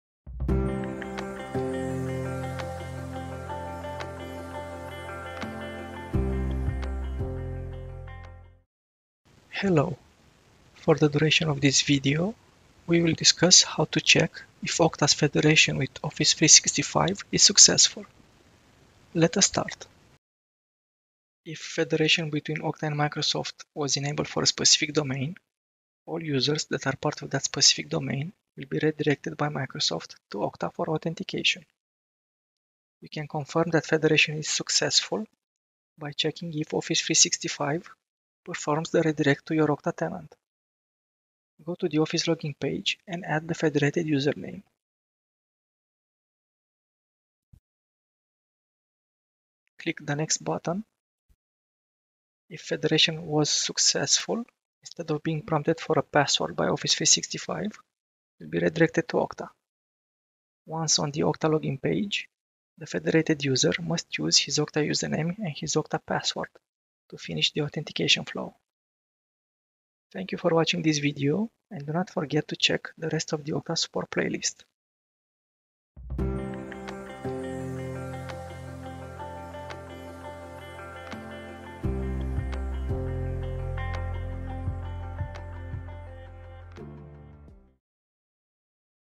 Confirm Okta Successfully Federated Office 365 | Okta Support
Learn how to confirm Okta successfully federated Office 365.